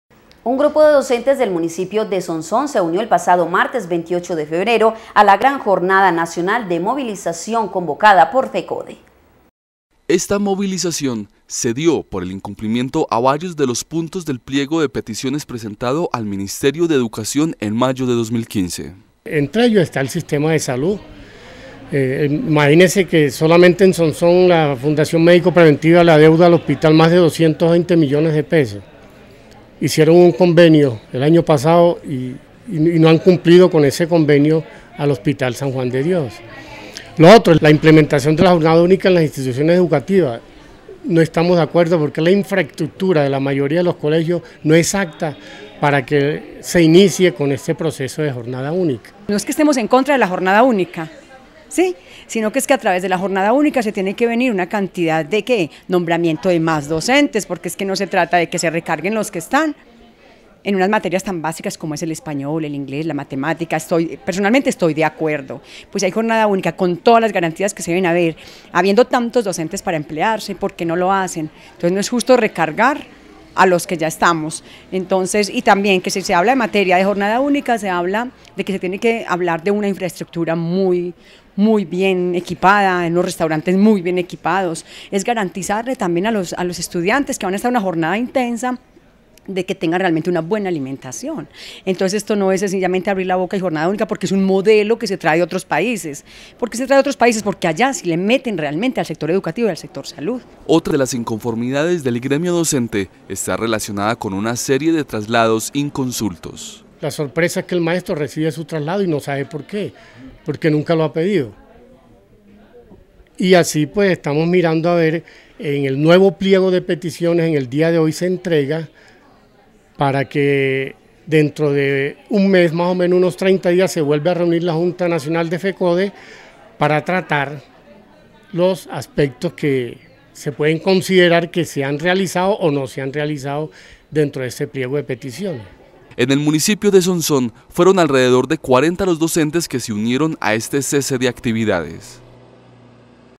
DOCENTES REALIZARON CESE DE ACTIVIDADES
Un grupo de docentes del municipio de Sonsón se unió el pasado martes 28 de febrero a la Gran Jornada Nacional de Movilización convocada por Fecode.